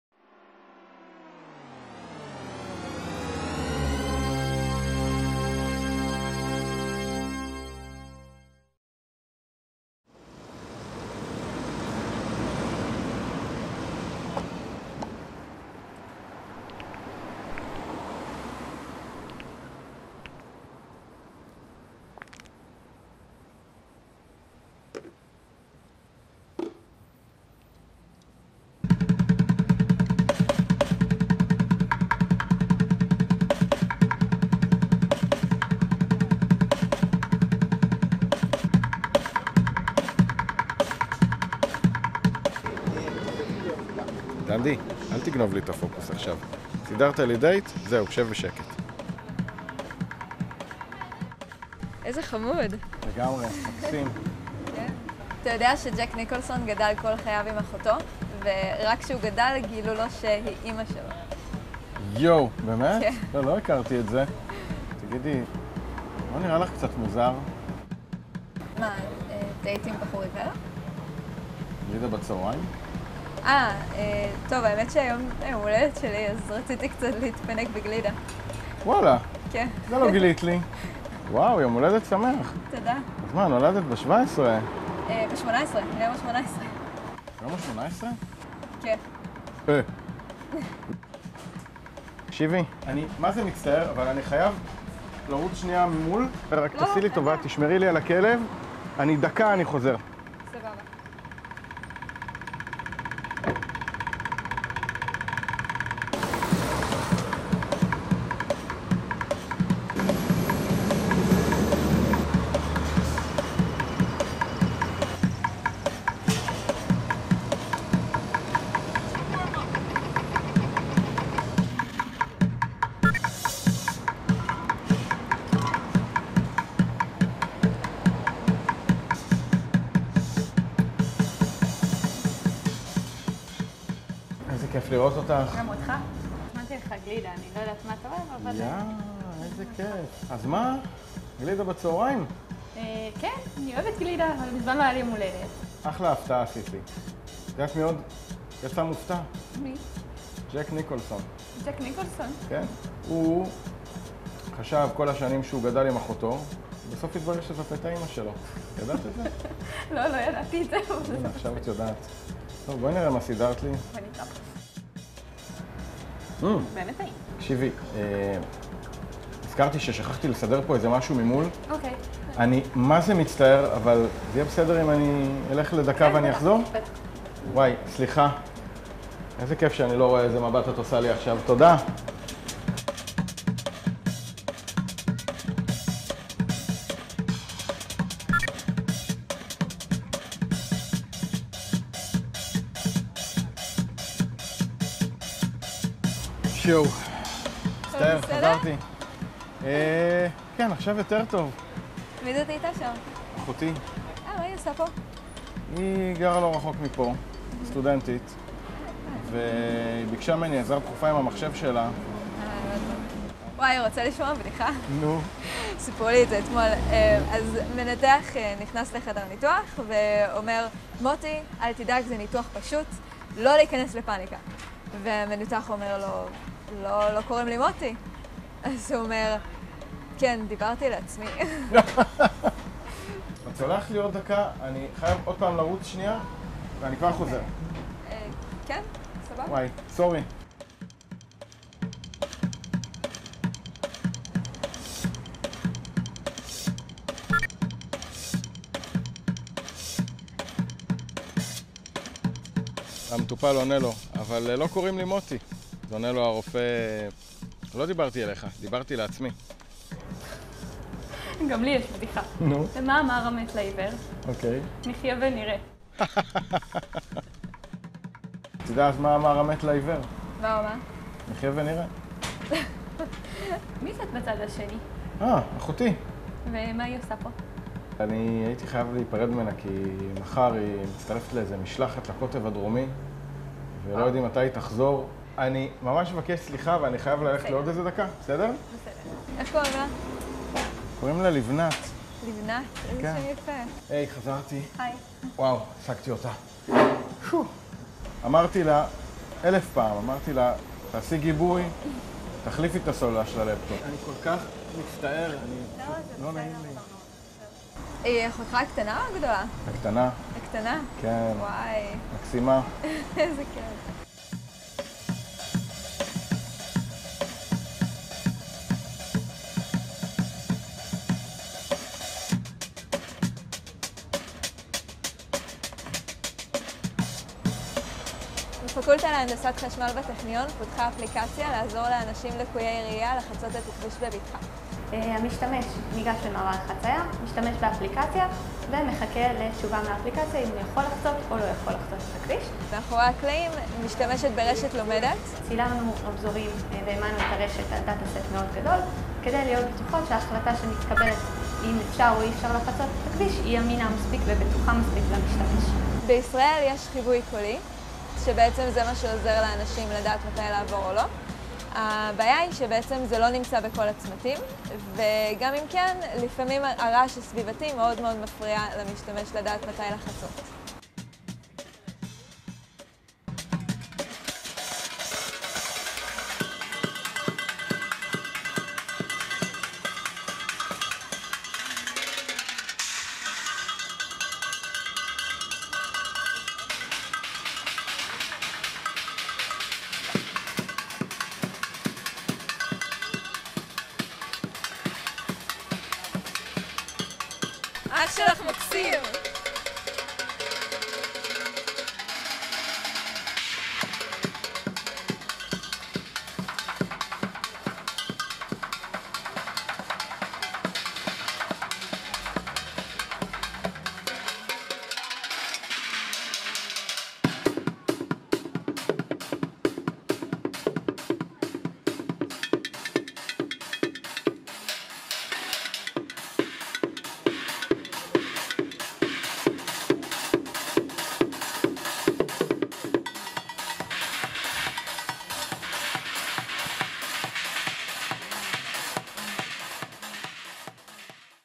Double Date (Pedestrian Traffic Light Detection App)
Crossing a road is a dangerous activity for pedestrians and therefore pedestrian crossings and intersections often include pedestrian-directed traffic lights. These traffic lights may be accompanied by audio signals to aid the visually impaired. In many cases, when such an audio signal is not available, a visually impaired pedestrian cannot cross the road without help. In the Technion, we developed an app that may help visually impaired people by detecting pedestrian traffic lights and their state (walk/don’t walk) from video taken with a mobile phone camera.

Undergraduate students: Dolev Ofri, Roni Ash
Assisting programmer: Ayellet Cohen
Supervised by: Yair Moshe
Screenplay, directing, filming and editing: Elad Keidan

Signal and Image Processing Laboratory (SIPL)
Andrew and Erna Viterbi Faculty of Electrical Engineering
Technion – Israel Institute of Technology

Thanks to all the people who took part in the making of this film:
בהשתתפות: עדי נתן, דולב עפרי, רונן אש, דנדי
בהופעת אורח: אבי רוזן
תסריט, בימוי, צילום ועריכה: אלעד קידן, סיוון סתווי
עזרו בהפקה: יאיר משה, מתן מליונר, איריס ברג
תודה מיוחדת: לימור – גלידריה נוצ'ולה, חמאדה – גלידריה לג'נדה

תודה מיוחדת לפרופ' איתן אלטמן על הרעיון לפרויקט